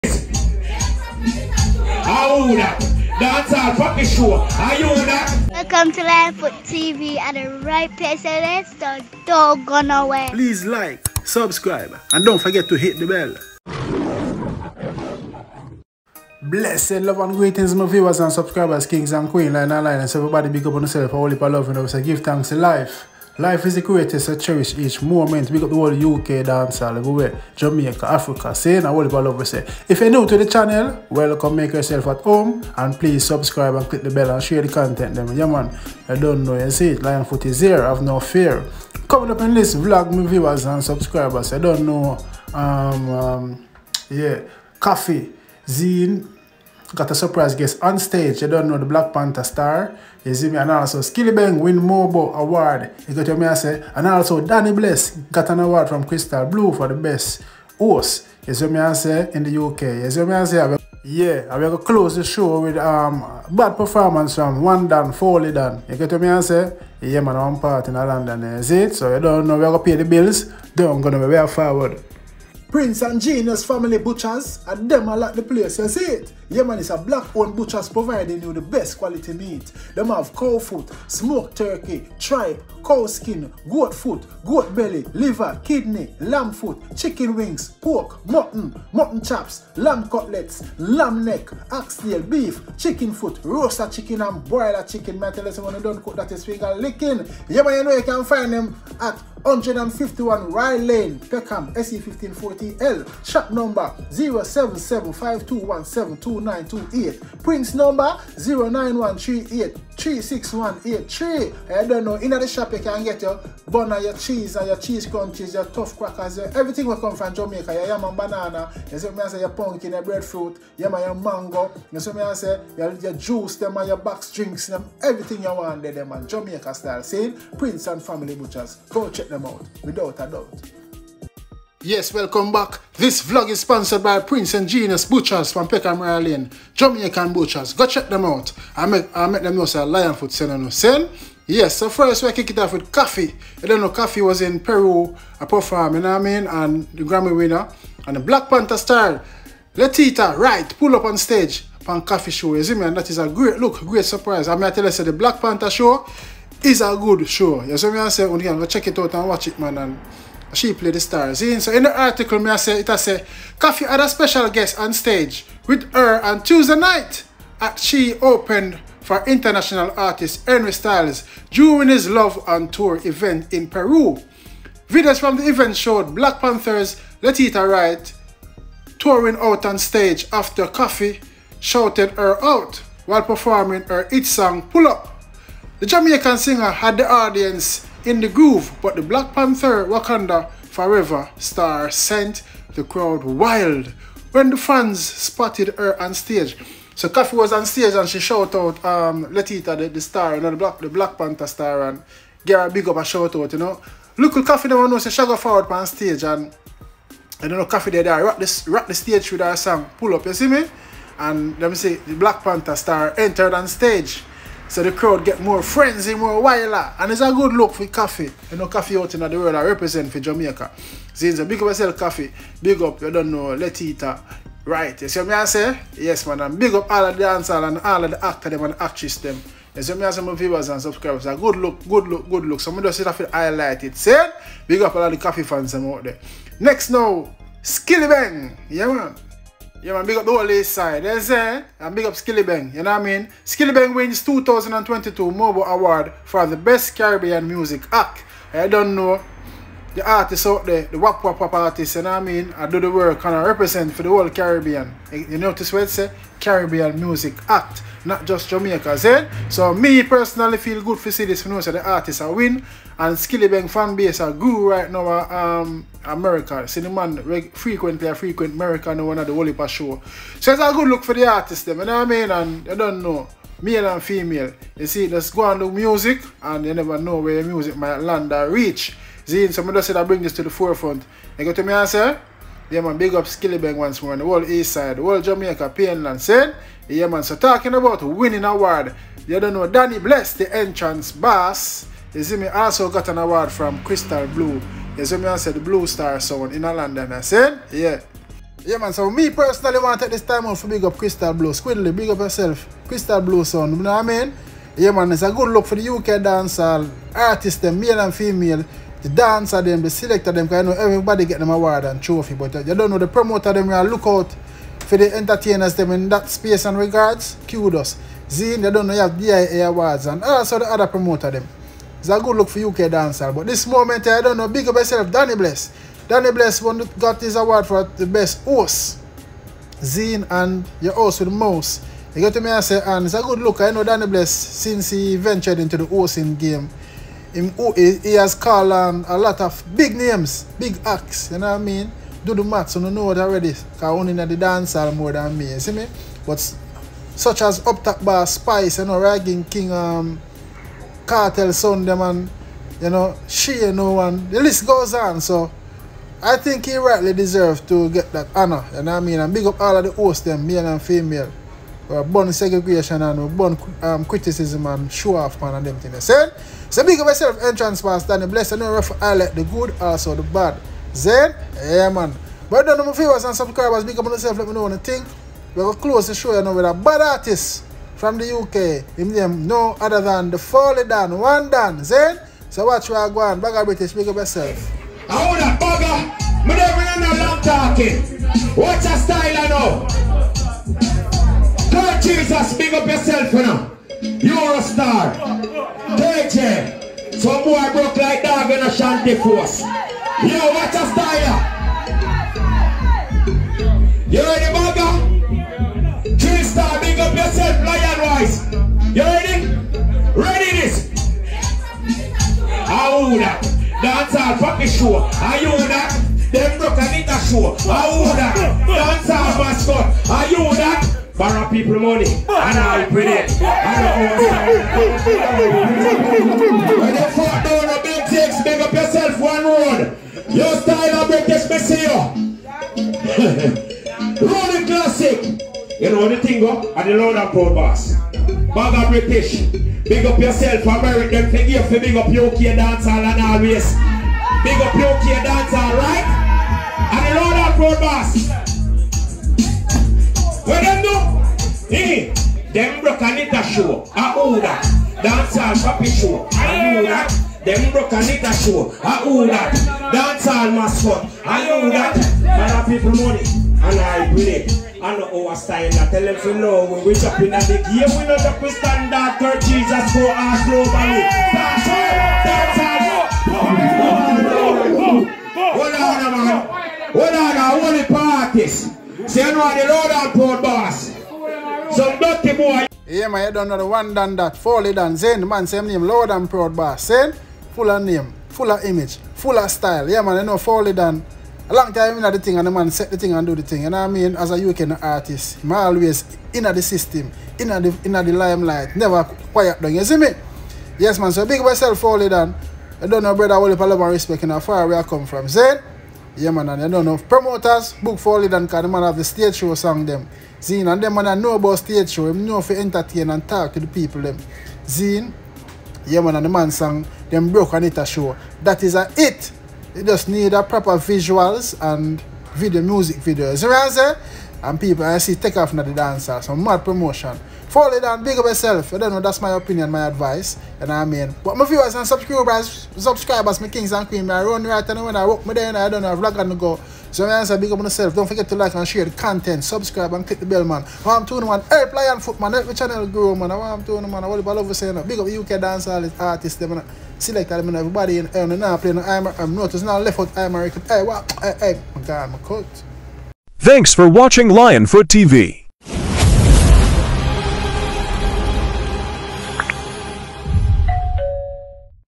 Koffee Got Surprised On Stage/ Skillibeng Won MOBO Award/ Danny Bless Won Best Host/ Fowlie Don Live
Blessed love and greetings.